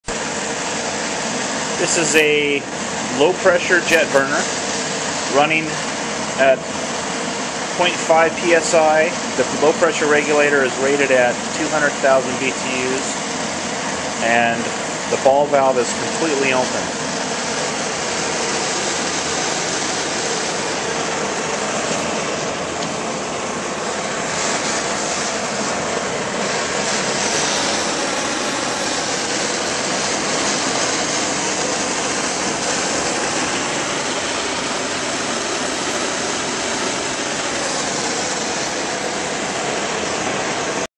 Jet Burner with Low Pressure Regulator
This video shows a low pressure regulator running a low pressure jet burner. The regulator is a Mr. Heater brand .5psi 200,000btu (low pressure) It is connected to a 175,000btu jet burner.